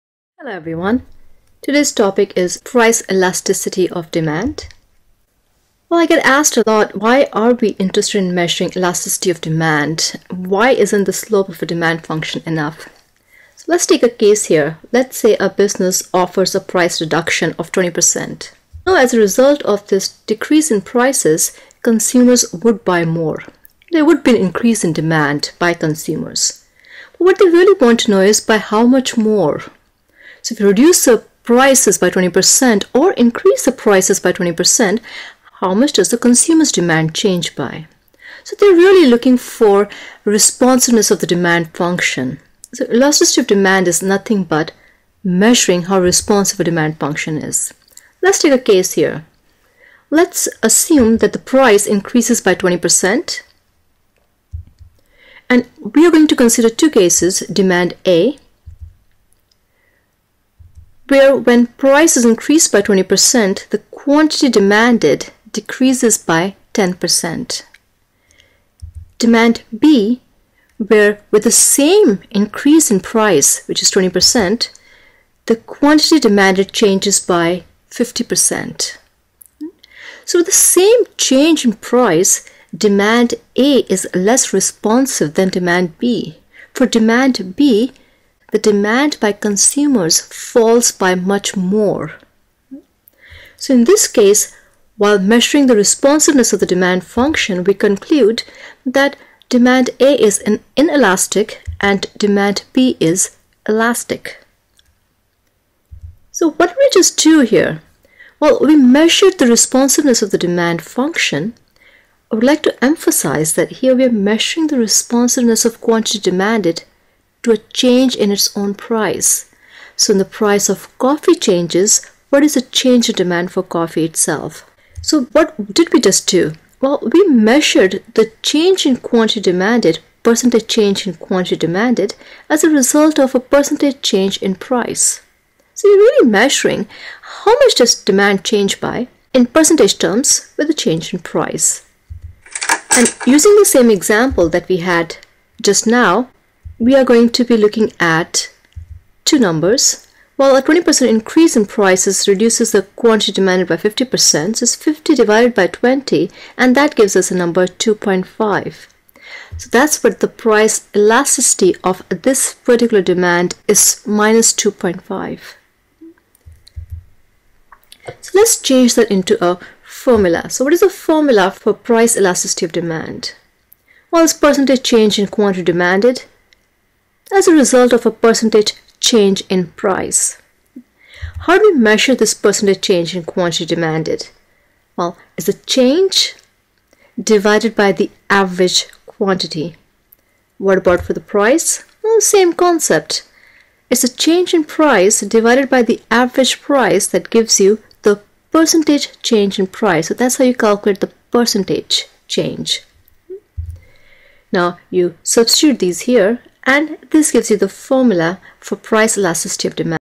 Elasticity - Price elasticity of Demand - What?
So, what's the elasticity of a linear demand function?
How do you measure it?